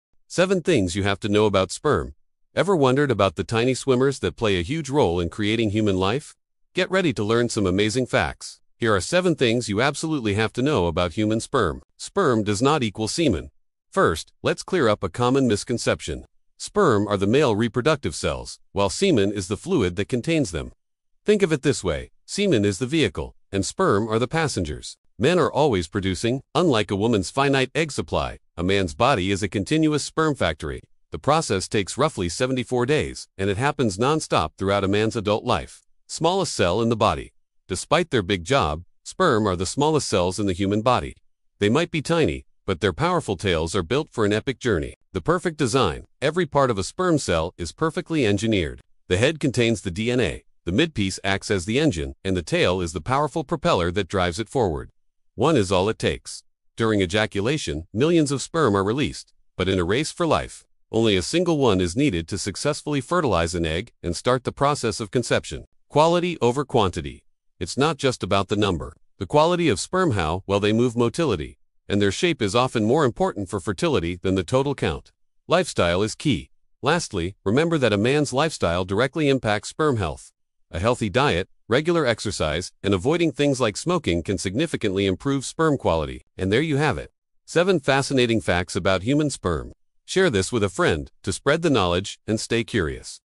The Sperm Fact That Will SHOCK You
Here are 7 mind-blowing facts about human sperm! From how they're produced to why a healthy lifestyle matters, this video will teach you everything you didn't learn in biology class.